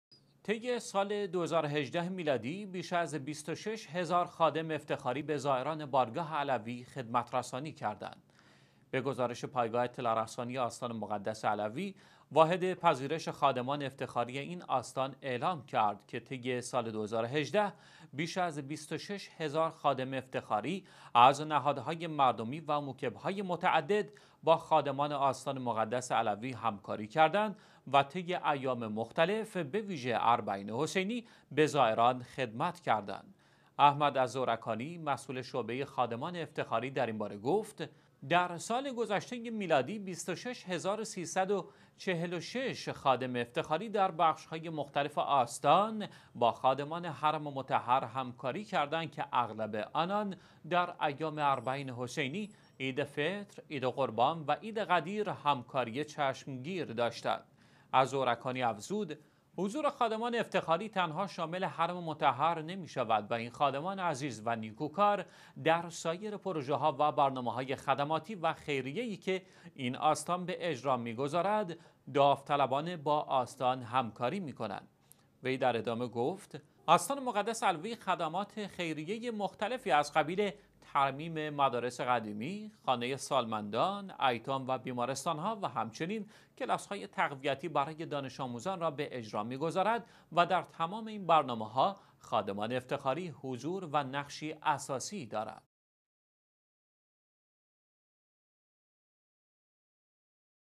حضور ده ها هزار خادم افتخاری در آستان مقدس علوی، طی سال گذشته میلادی
طی سال 2018 میلادی،  بیش از 26 هزار خادم افتخاری  به زائران بارگاه علوی خدمت رسانی کردند.
به گزارش پایگاه اطلاع رسانى آستان مقدس علوى، واحد پذیرش خادمان افتخاری این آستان اعلام کرد: طی سال 2018  بیش از 26 هزار خادم افتخاری از نهادهای مردمی وموکب های متعدد، با خادمان آستان مقدس علوی همکاری کردند و طی ایام مختلف، به ویژه اربعین حسینی، به زائران خدمت کردند.

«احمد الزوركانى» مسئول شعبه خادمان افتخارى، در اين باره گفت: در سال گذشته ی میلادی،  26346 خادم افتخاری در بخش های مختلف آستان، با خادمان حرم مطهر همکاری کردند که اغلب آنان در ایام اربعین حسینی، عید فطر، عید قربان و عید غدیر، همکاری چشم گیر داشتند.

الزورکانی افزود: حضور خادمان افتخاری تنها شامل حرم مطهر نمی شود و این خادمان عزیز و نیکوکار، در سایر پروژه ها و برنامه های خدماتی و خیریه ای که این آستان به اجرا می گذارد،  داوطلبانه با آستان همکاری می کنند.

وی در ادامه گفت: آستان مقدس علوی خدمات خیریه ی مختلفی از قبیل:  ترمیم مدارس قدیمی، خانه سالمندان، ایتام،  و بیمارستان ها، و همچنین کلاس های تقویتی برای دانش آموزان را به اجرا می گذارد و در تمام این برنامه ها خادمان افتخاری حضور و نقشی اساسی دارند.